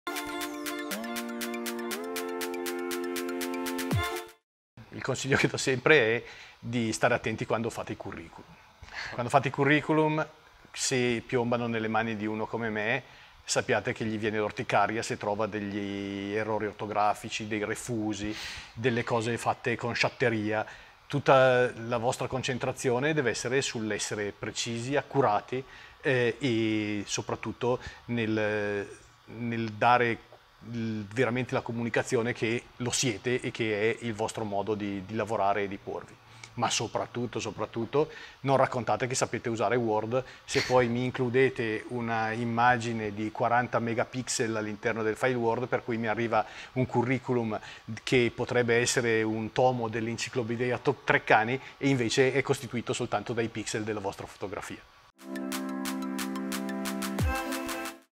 Marco Deligios e l'importanza di un curriculum vitae curato
Quali errori dobbiamo evitare nella stesura del nostro 📄 CV❓ A cosa dobbiamo prestare più attenzione❓ Ascolta i consigli di Marco Deligios 💪🏼